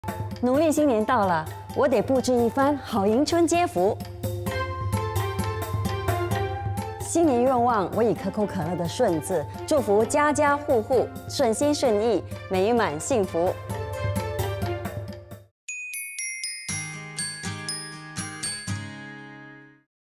Coca Cola CNY TVC 20s - Featuring Xiang Yun 向云
Client: Coca Cola SG
Objective: CNY 2015 TVC 20s
Featuring Xiang Yun 向云
Platform: TVC
Language: Mandarin